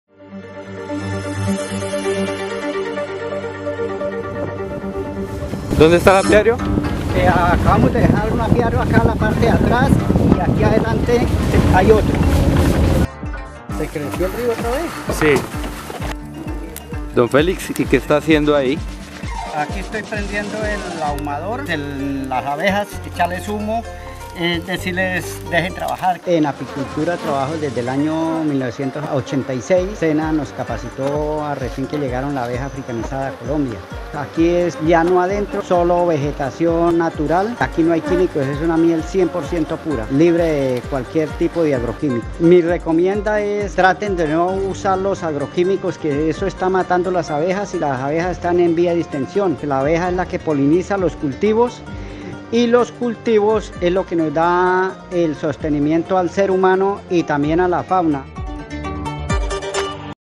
Colombee - Miel de árbol de Acacia Mangium, desde el corazón de Colombia. 🐝🍯❤️🏞️🇨🇴
Colombee: Personas, abejas y medio ambiente cómo una sola familia…
Somos una empresa Colombiana que por más de 10 años se ha dedicado a la producción y comercialización de productos de la colmena, en especial de miel de abejas 100% natural.